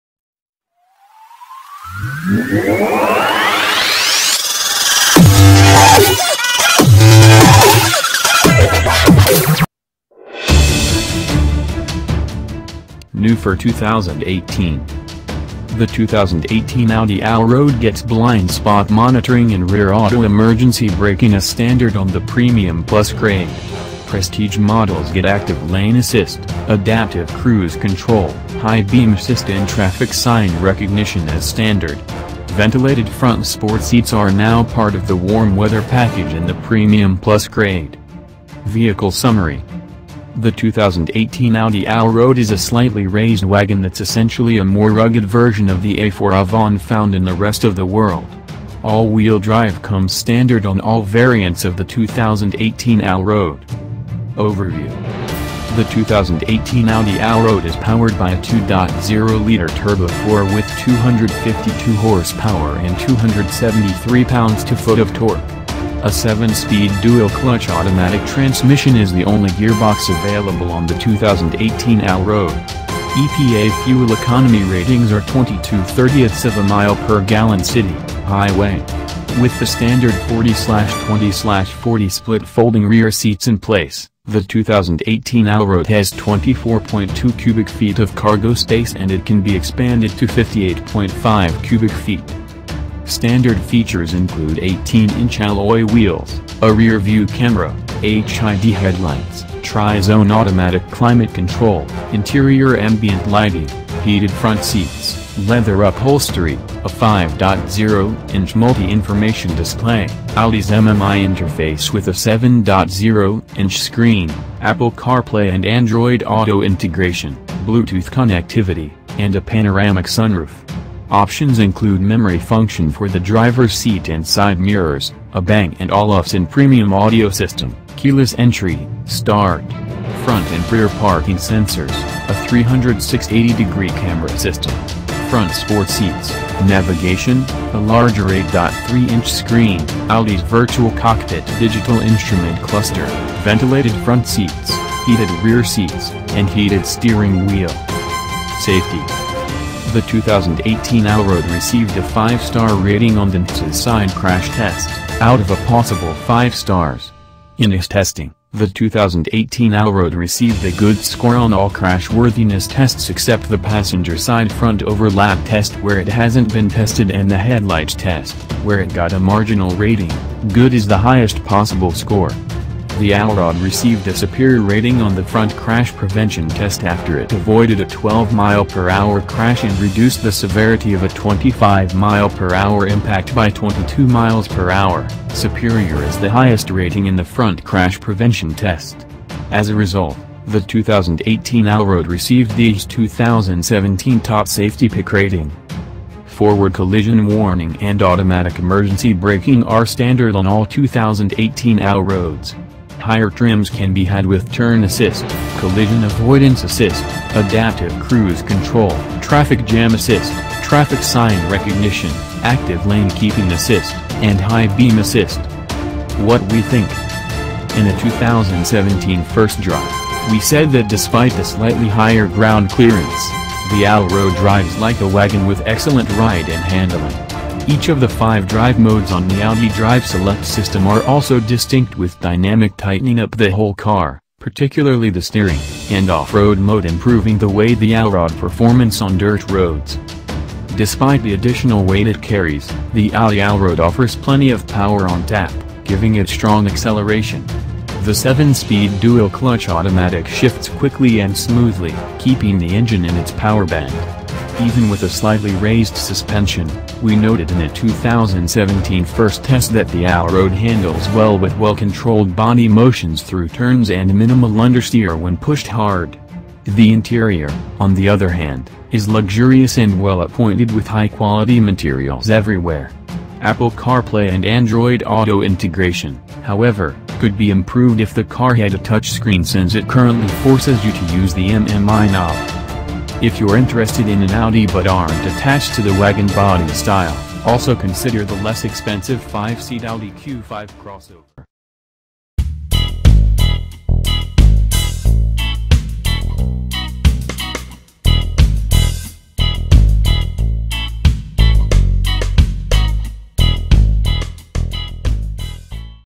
[HOT NEWS]!!! 2018 Audi A4 allroad quattro – PJYT002
Takes to all roads
The road can be the start of a new adventure, so it’s important to have a companion that won’t hold you back. The Audi A4 allroad® sports a 252-hp 2.0-liter TFSI® engine paired with a seven-speed S tronic® dual-clutch automatic transmission to create a dynamic, comfortable and sporty driving experience that can’t wait to come along for the ride.